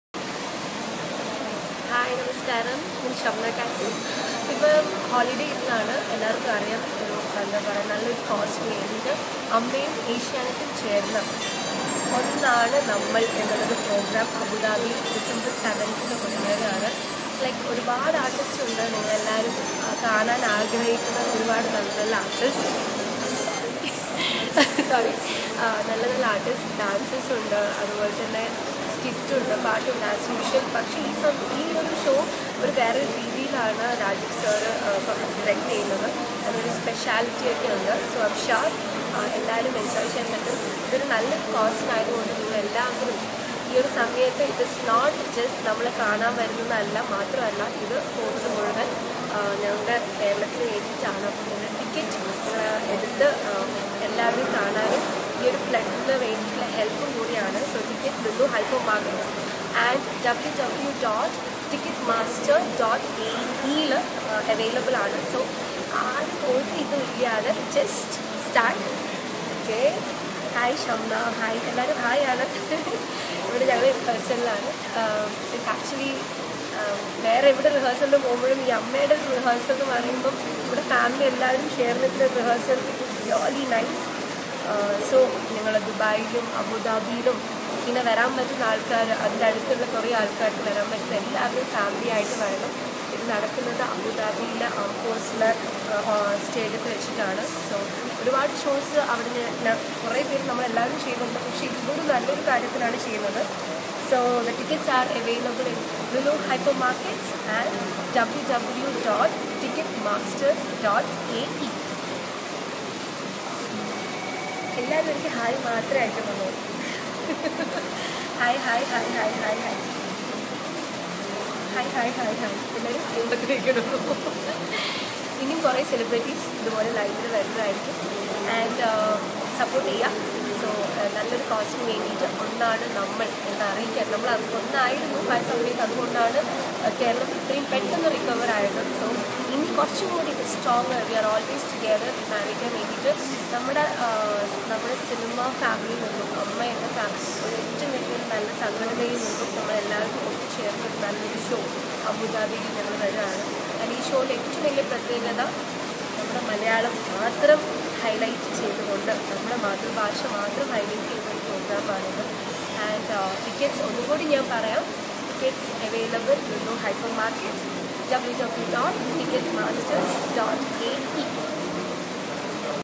Shamna Kasim Live || Onnanu Nammal || Asianet - AMMA Show || Dec7 Evening @ AFOC - ABU DHABI
Onnanu Nammal || Asianet - AMMA Show || Flood Relief || December 7 Evening @ Armed Forces Officers Club - ABU DHABI

and Lulu Hyper Markets